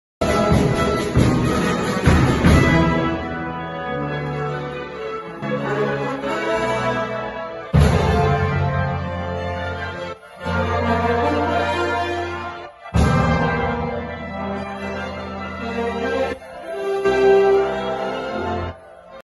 En el Museo de Arte, se realiza el Concierto de música tradicional mexicana
En San Pedro Museo de Arte, se realiza el Concierto de música tradicional mexicana del Conservatorio de Música de Puebla en el marco del 201 Aniversario de la consumación de la Independencia.